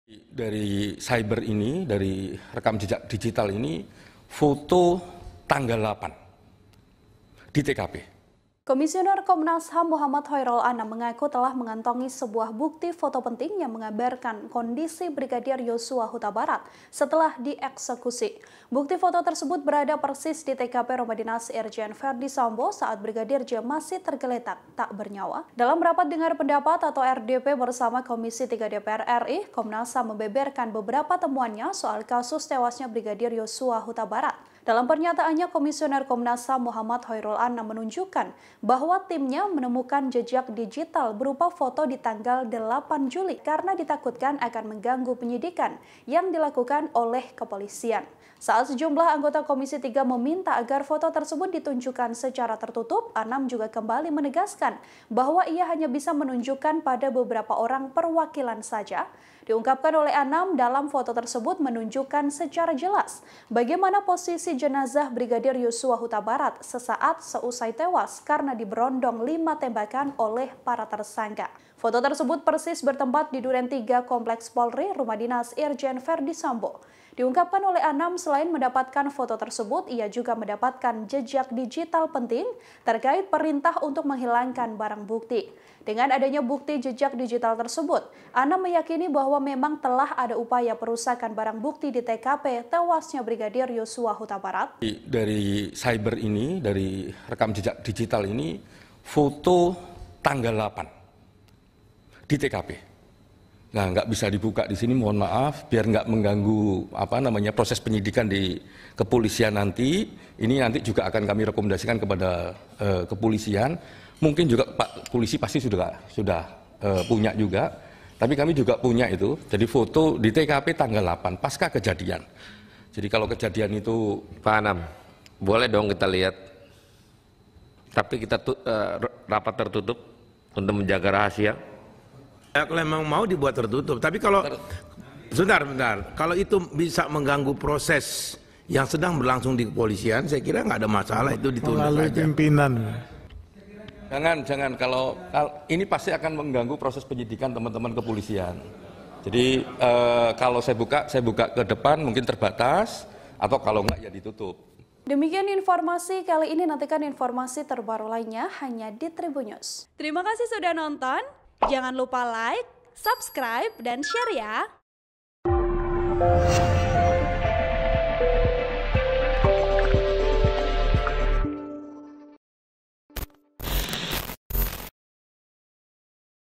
Komnas HAM Ungkap Foto Penting di TKP Tampilkan Posisi Brigadir J seusai Tewas Ditembak Mati
TRIBUN-VIDEO.COM - Komisioner Komnas HAM Mohammad Choirul Anam mengaku telah mengantongi sebuah bukti foto penting yang menggambarkan kondisi Brigadir Yosua Hutabarat setelah dieksekusi.

Bukti foto tersebut berada persis di TKP rumah dinas Irjen Ferdy Sambo saat Brigadir J masih tergeletak tak bernyawa.

Dalam rapat dengar pendapat (RDP) bersama Komisi III DPR RI, Komnas HAM membeberkan beberapa temuannya soal kasus tewasnya Brigadir Yosua Hutabarat.